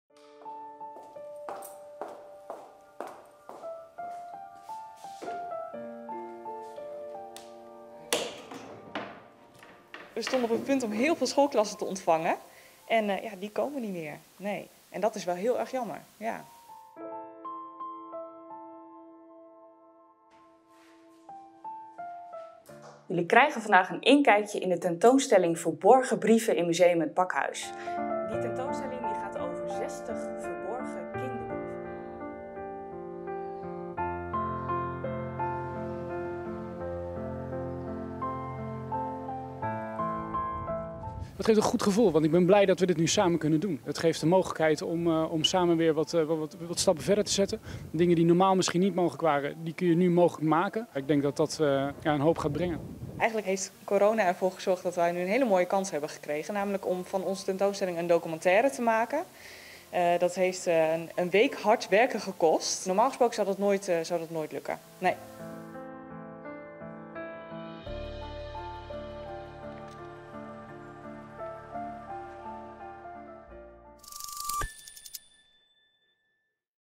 Corona geeft museum in Ermelo unieke kans
ERMELO - Het is een moeilijke tijd voor musea. Want die mogen voorlopig nog niet open. Maar in Ermelo dachten ze: als mensen niet naar ons museum mogen komen, dan brengen we het museum naar de mensen. Museum Het Pakhuis had net de nieuwe tentoonstelling 'Verborgen Brieven' ingericht, speciaal voor 75 jaar vrijheid. En toen werd alles anders.